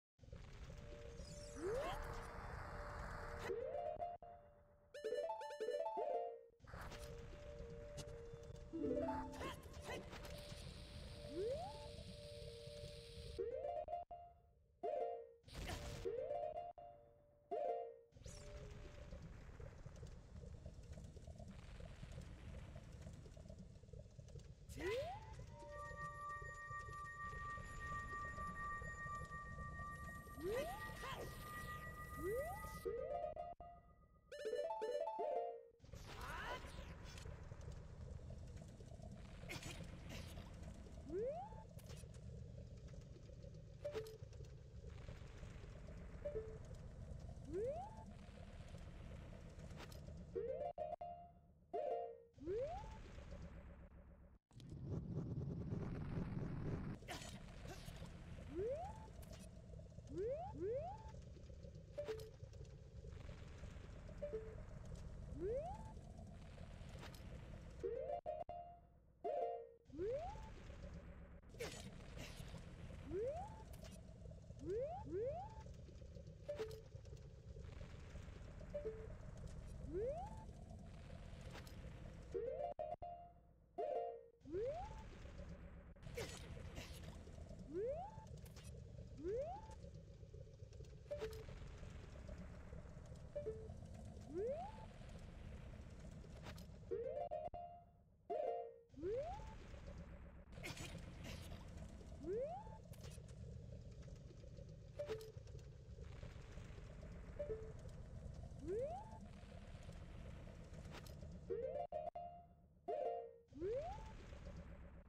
1080 Wrong Warp With Bomb OI (2 Frames to Hold Up)
I wanted to find a bomb OI setup with 2 frames to hold up and easy cues, but I couldn't get the angles to work out so this requires a nut throw to change angles. Unfortunately this adds a pause if you have sword.

There's 7 frames where releasing left will still land inside the door but only 4 frames work with the frame 1 and 2 adjustments shown here. Hopefully someone can come up with better ones.

0:00 OI Setup
0:22 Clip Setup
0:38 Jump Setup
0:57 Frame 1 Adjustment
1:11 Frame 2 Adjustment
1:26 Frame 3 Adjustment
1:40 Frame 4 Adjustment